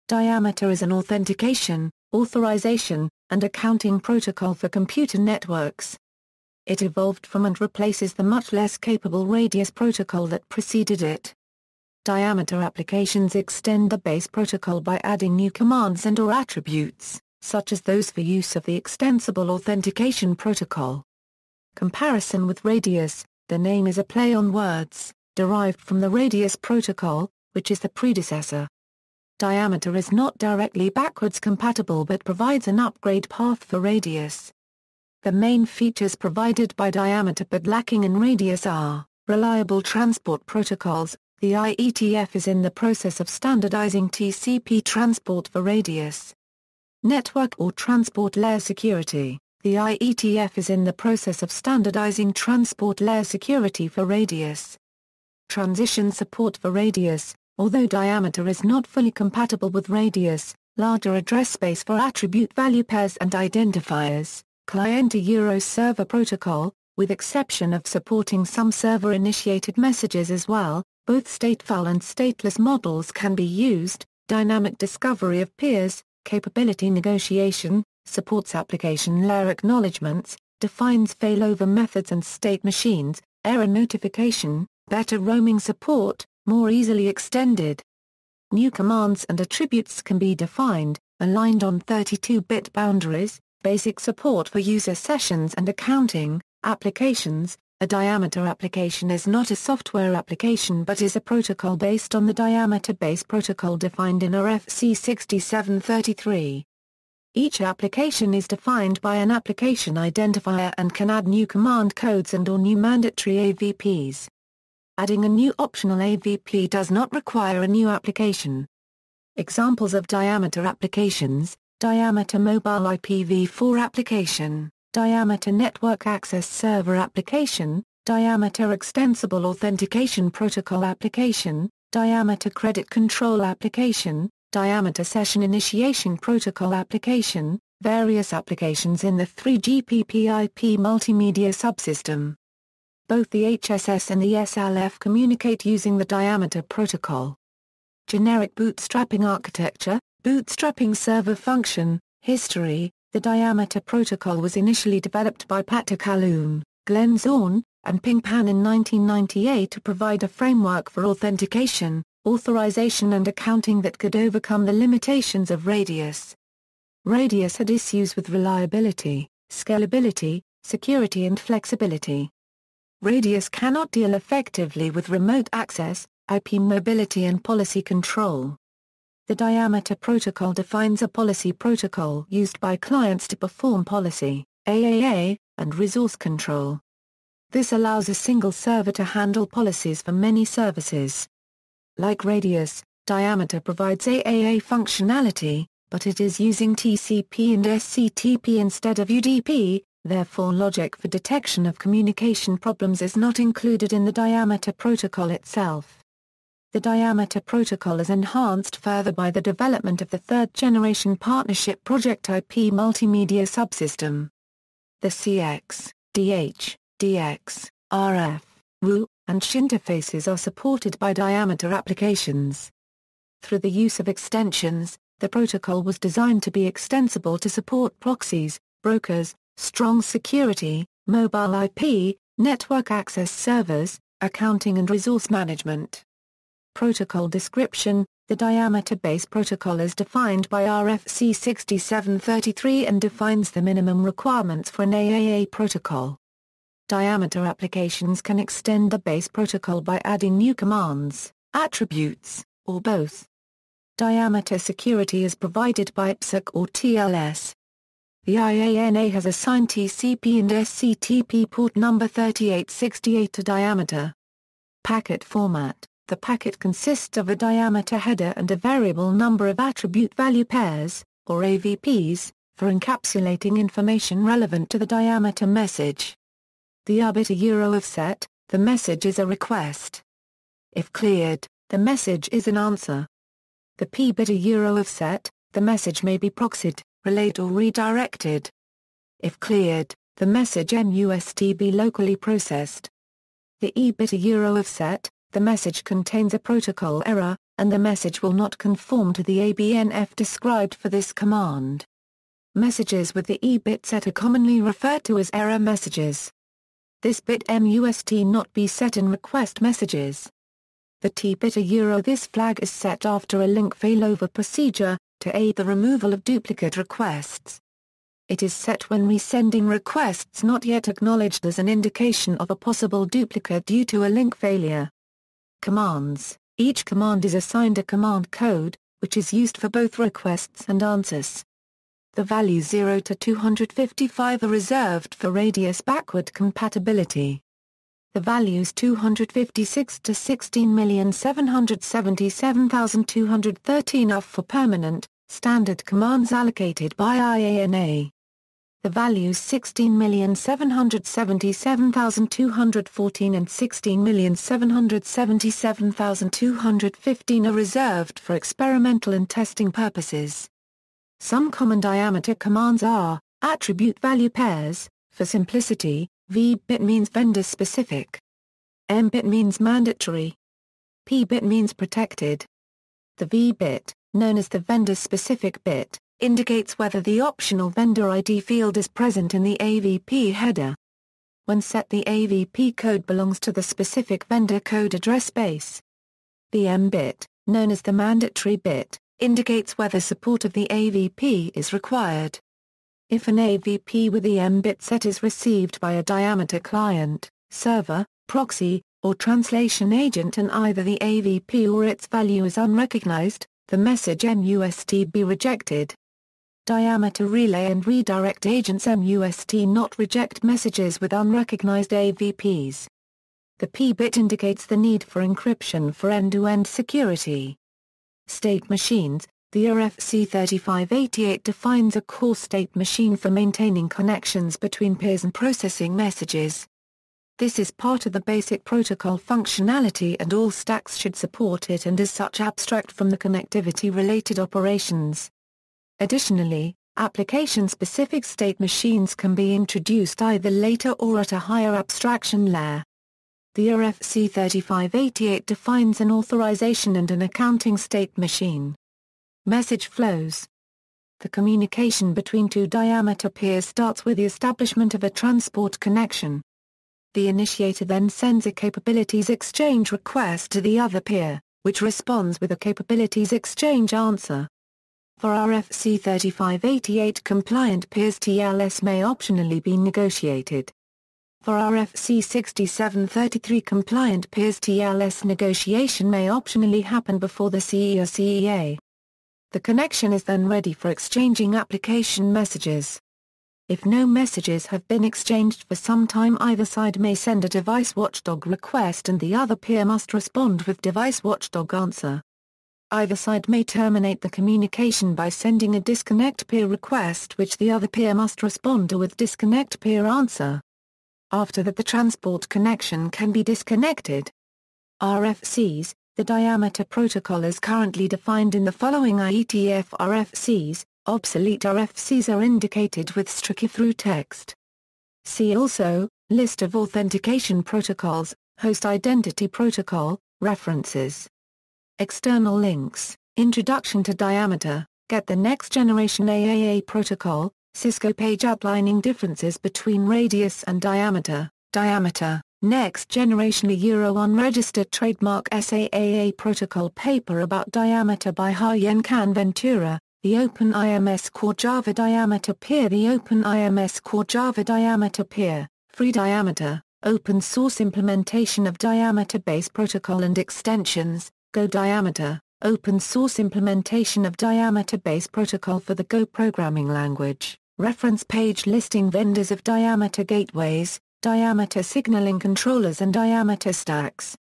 Diameter (protocol)
Diameter is an authentication, authorization, and accounting protocol for computer networks. It evolved from and replaces the much less capable RADIUS protocol that preceded it.
Diameter Applications extend the base protocol by adding new commands and/or attributes, such as those for use of the Extensible Authentication Protocol (EAP).